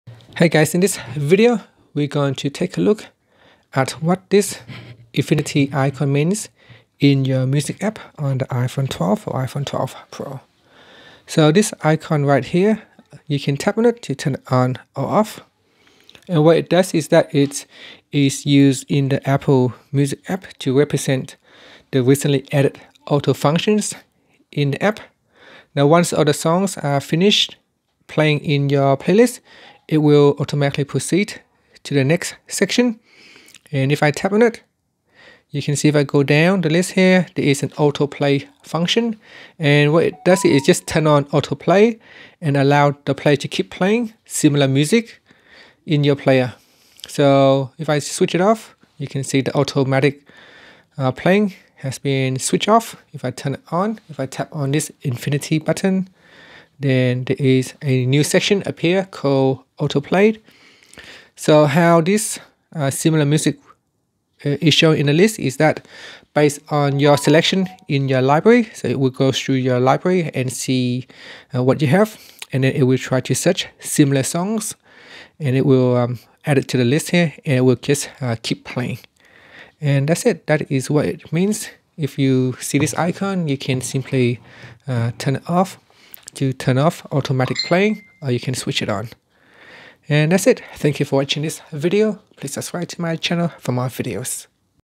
iPhone 12: What Does the Infinity Icon Mean in the Music App?
Learn what the infinity icon means in the Music app on the iPhone 12 / iPhone 12 Pro.